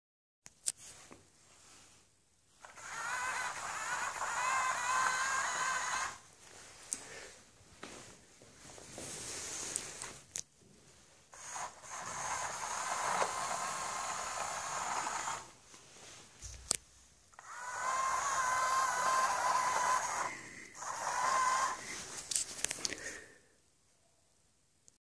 Carrinho com arduino e sensores ultra sons
Ao aproximar alguma coisa do carrinho ele afasta-se